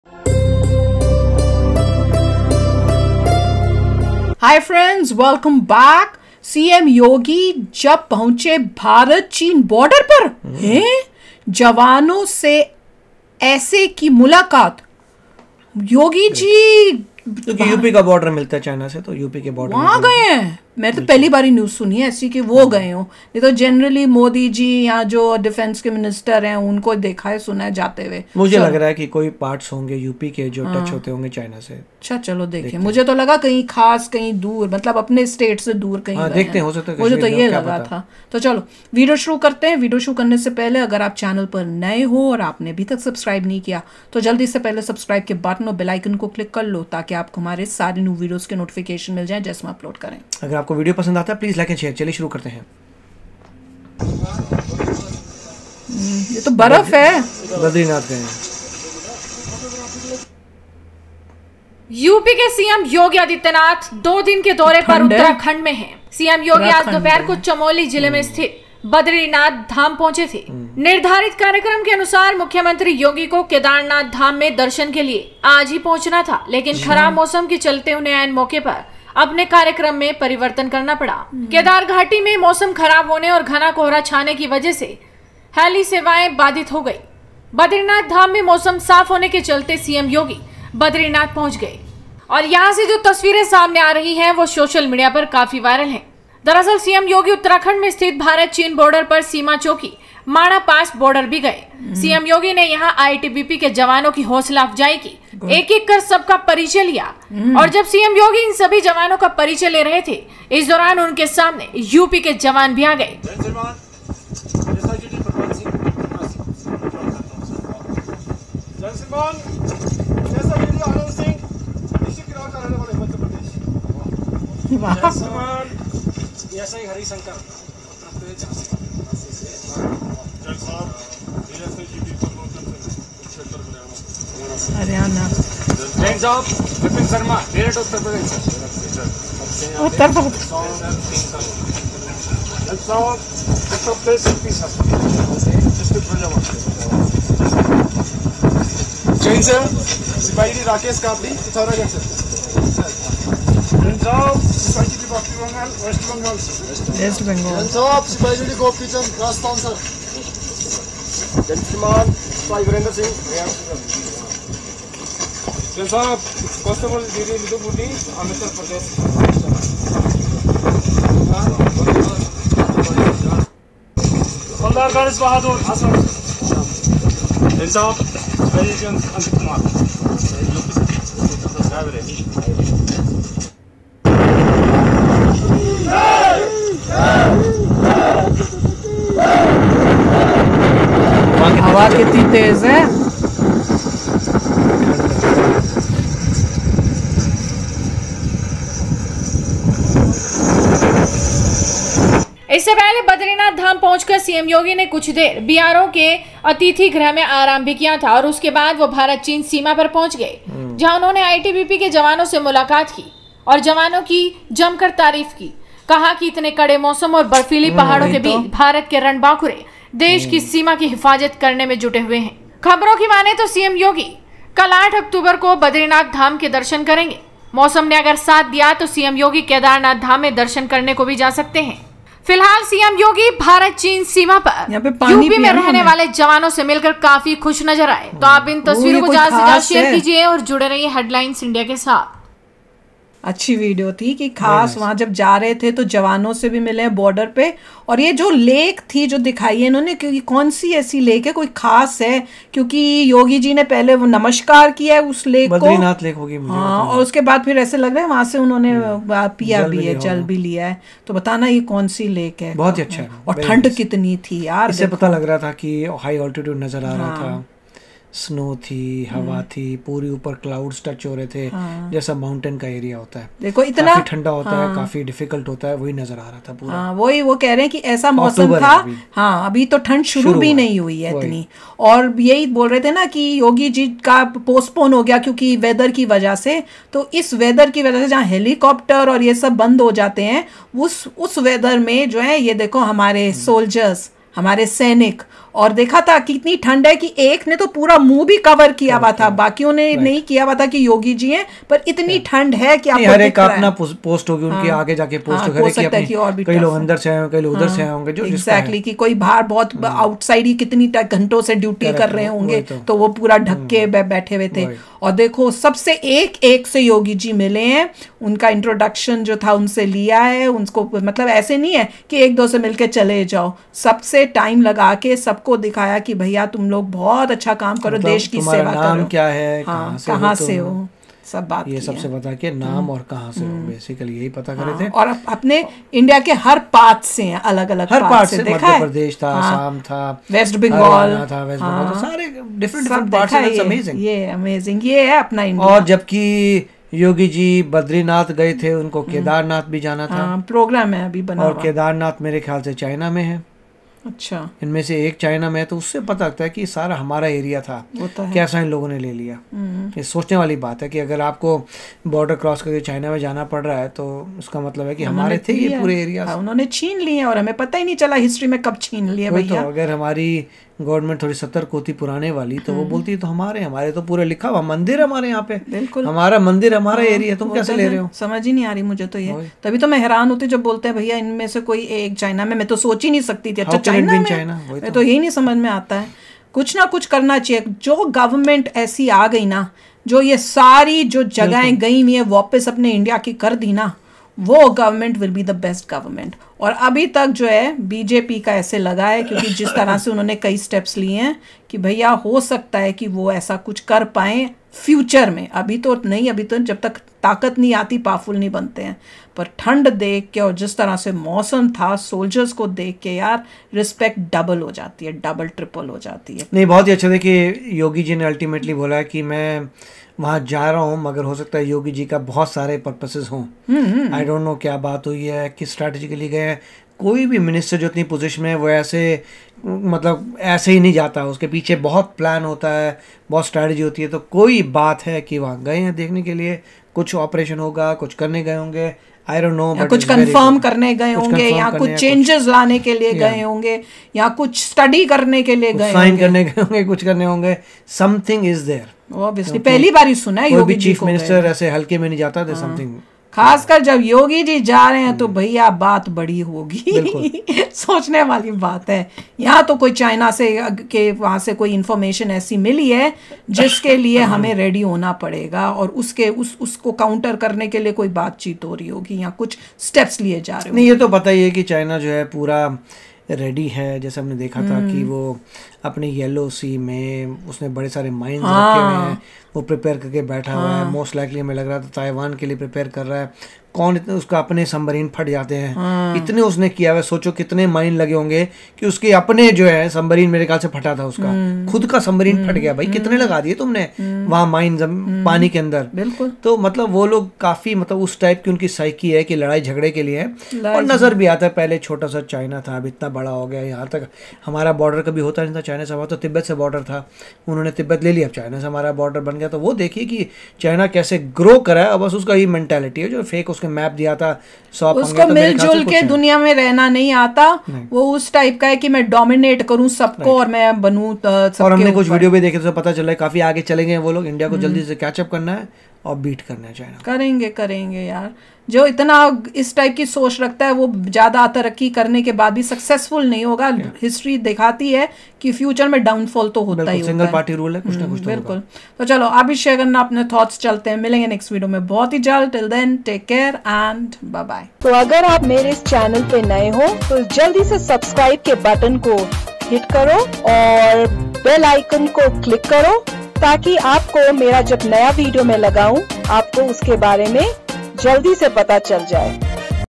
CM Yogi जब पहुंचे भारत-चीन बॉर्डर पर😲, जवानों से ऐसे की मुलाकात😱| Gajab Reactions !✨
Love & Enjoy!

channel_headlinesIndia